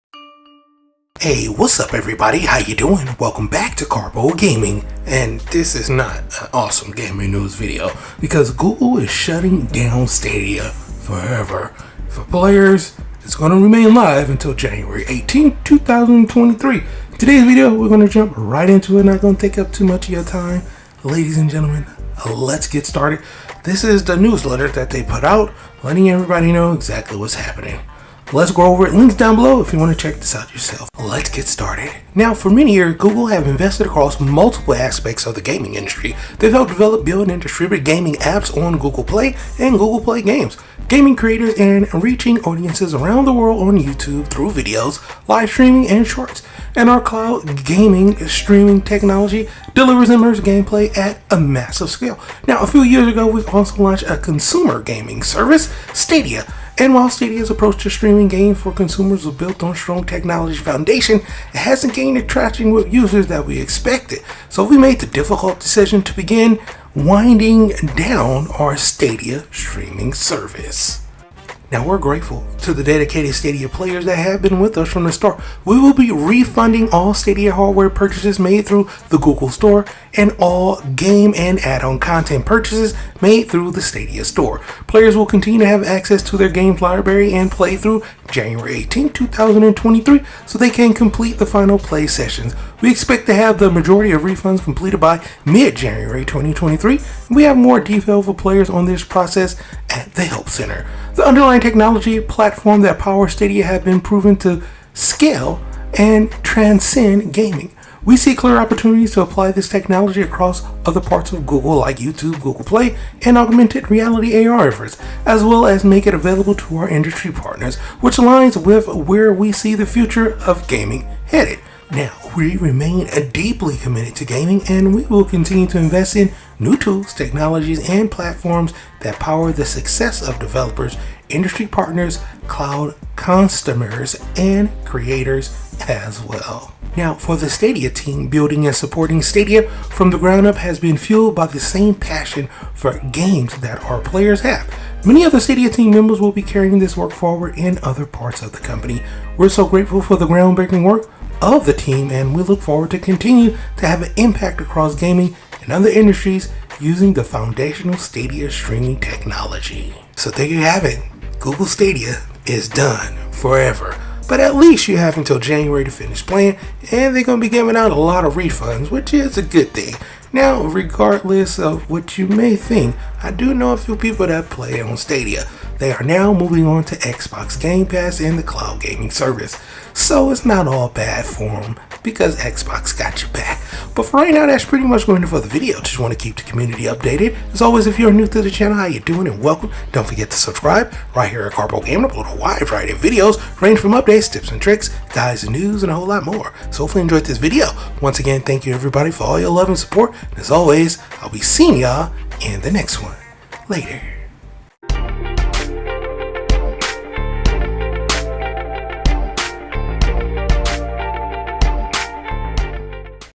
Google Stadia: Is Shutting Down FOREVER! Here's Everything We Know So Far
Google is Offically shutting down Stadia and its cloud gaming service. The service will remain live for players until January 18th, 2023. Heres everything we know so far.

Timestamps:
0:00​​ Intro
Outro